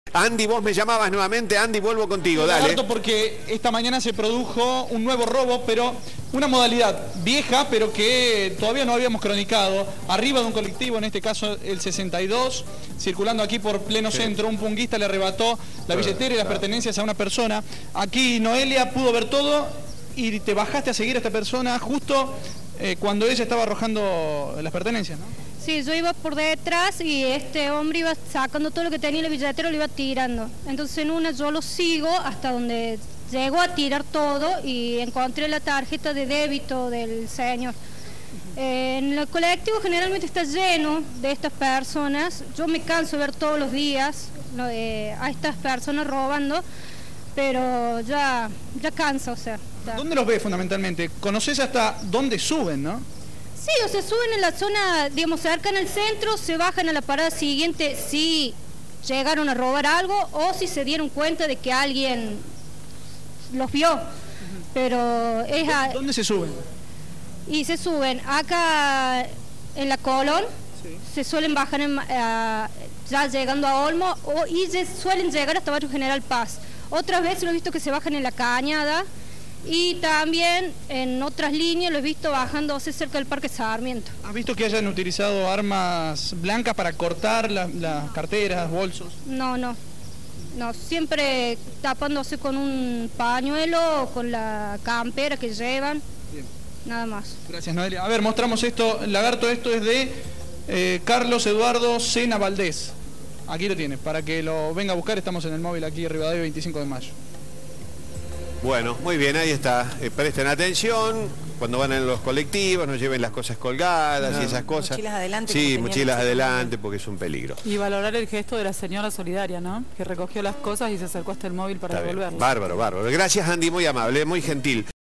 pasajera recupero pertenencias de un punguista 30 10 2014
Noelia, una vecina que viajaba en un colectivo, siguió a un punguista de la línea 62 y recuperó las pertenencias de un pasajero.